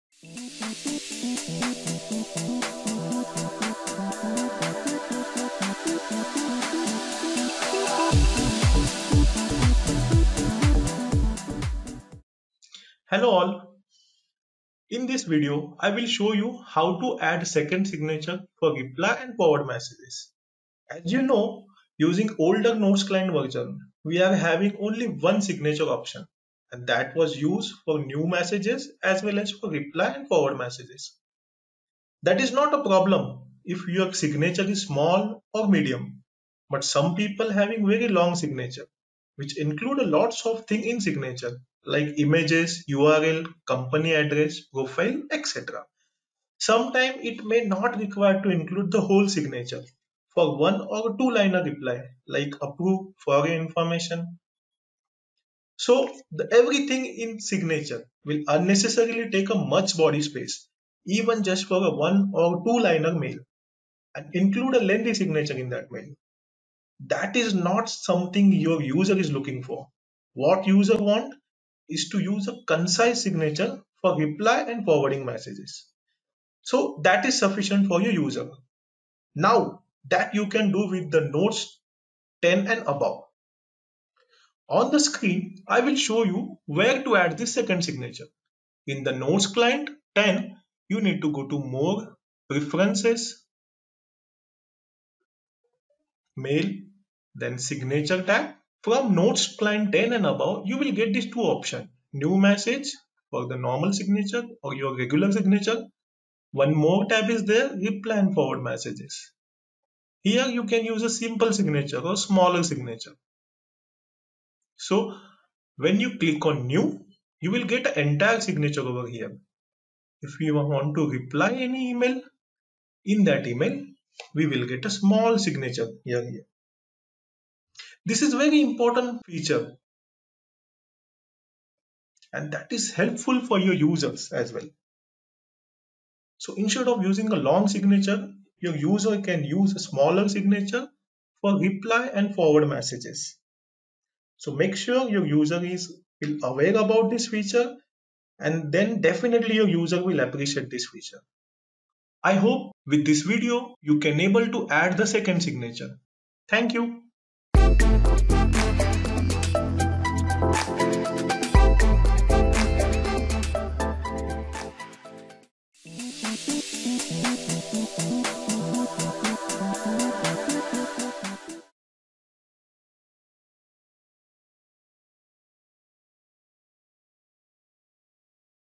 HCL Notes - Add a Second Signature in Notes Client
This video demonstrates how to add a second signature for replies and forwards in the Notes client. It is a new feature introduced in the V10 mail template.